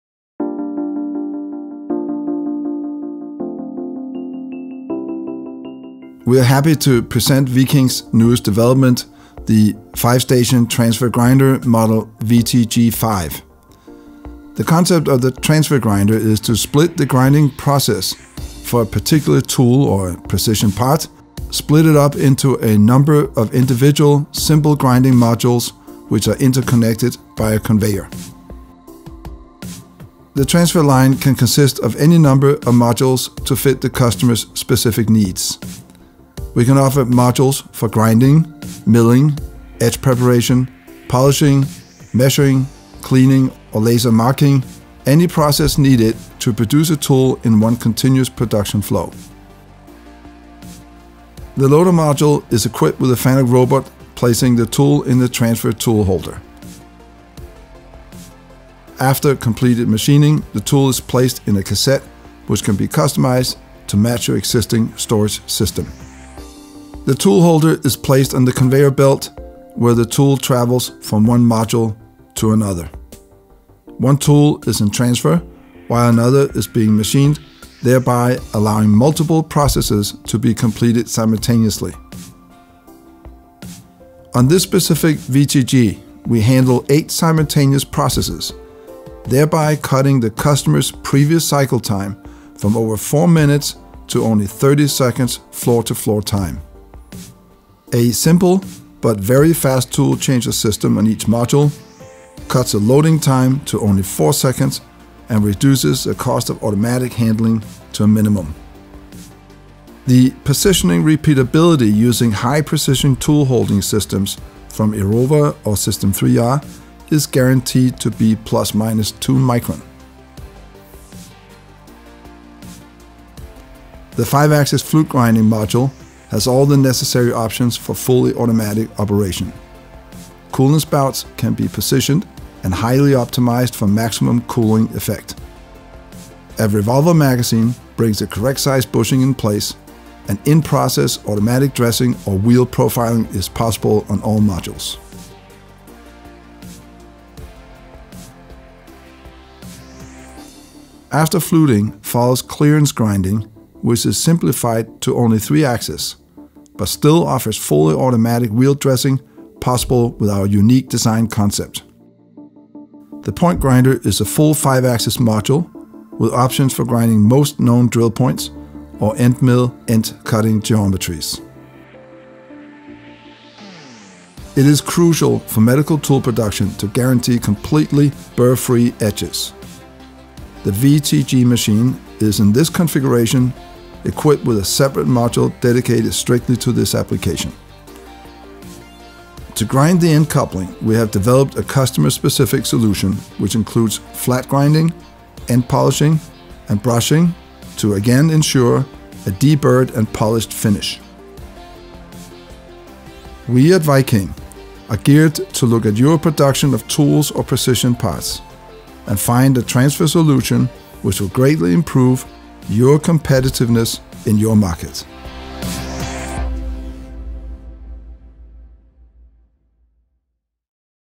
VIKING Transfer Grinder Model VTG - Technical Video
VIKING’s newest development, the 39 - axes CNC transfer grinder – Model VTG.